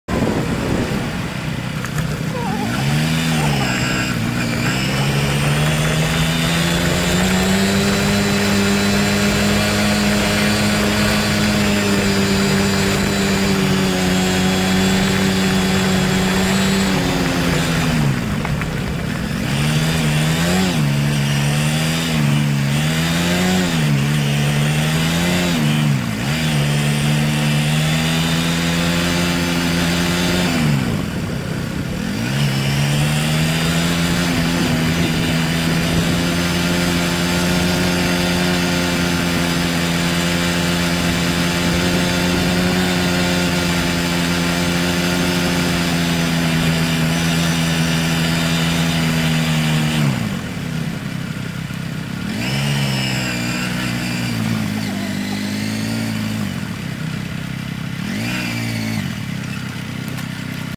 Edisto Beach State Park
A motorcycle (Honda Grom!) tour of the Live Oak Campground at Edisto Beach State Park in South Carolina.